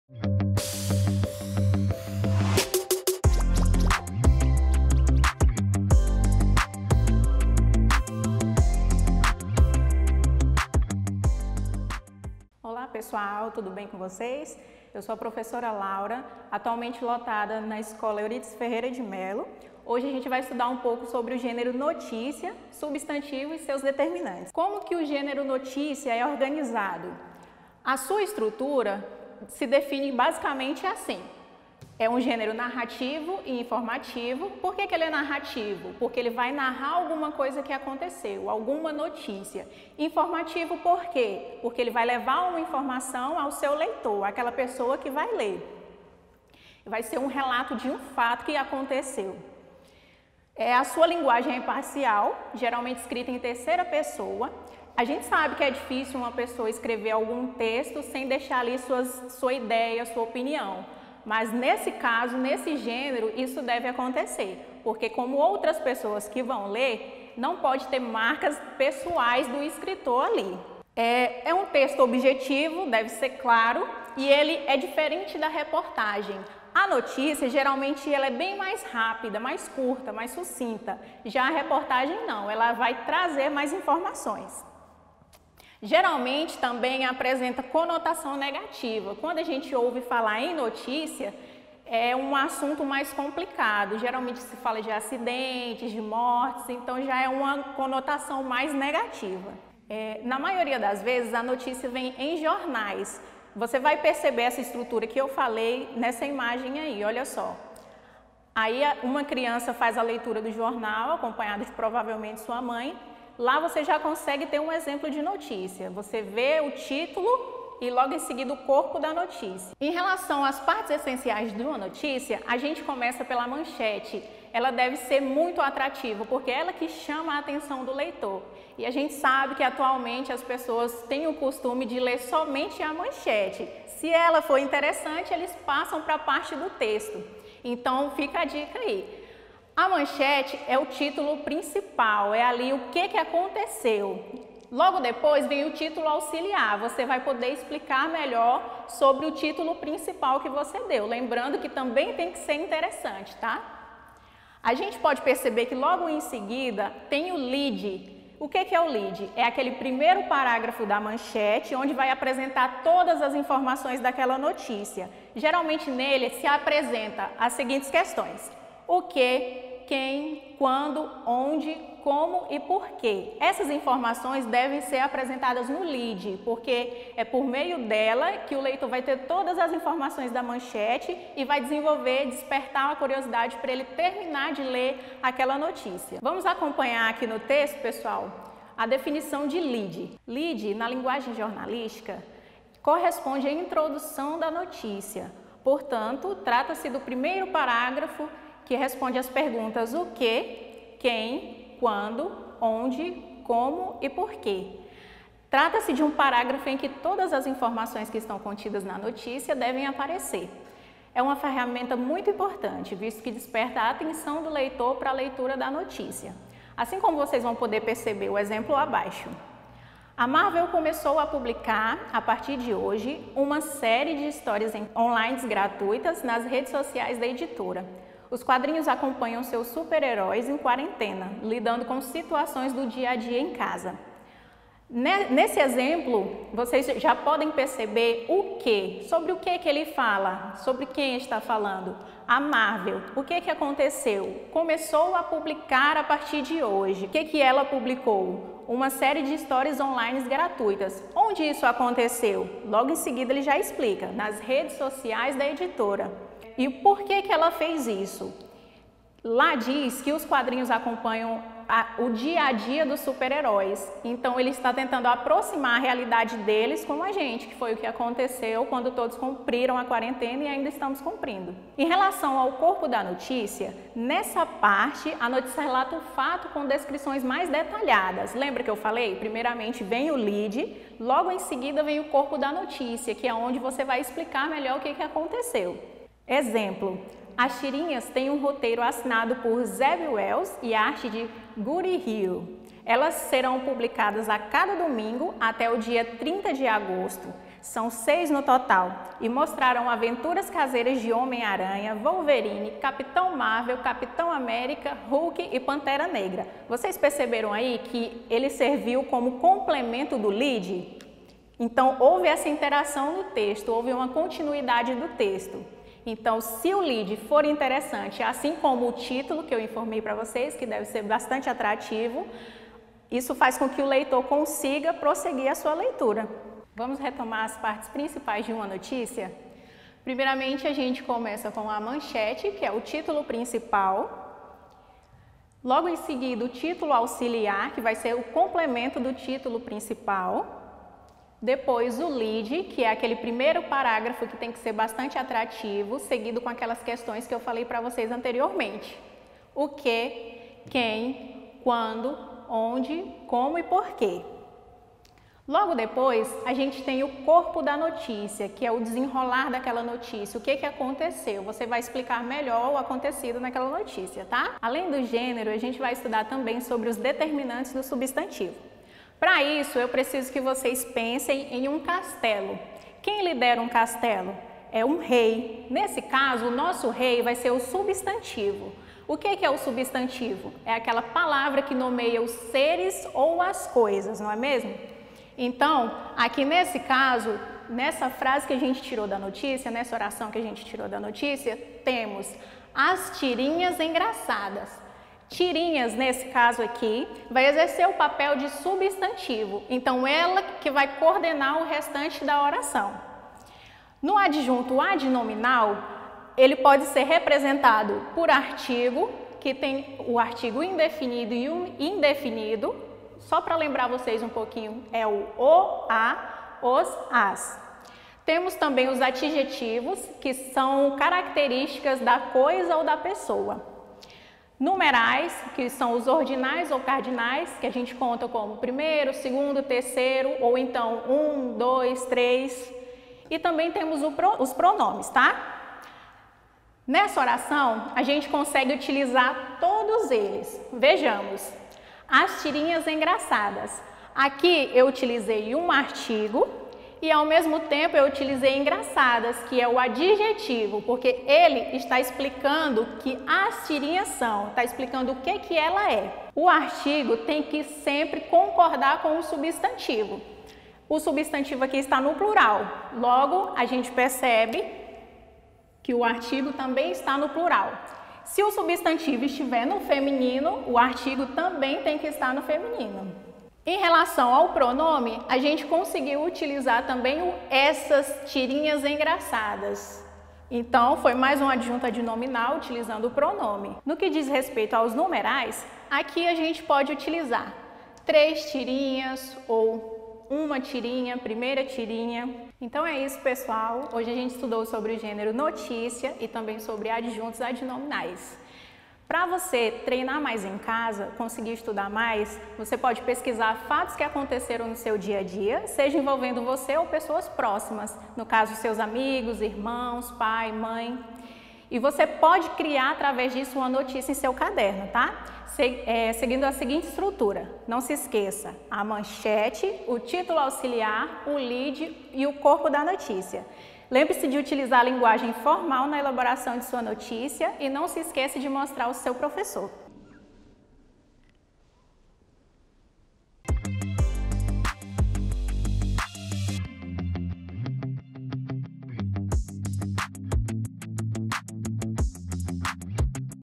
8º Ano - Língua Portuguesa - Profª. Laura Beatriz - Aula 05
Tema: Gênero notícia, substantivo e seus determinantes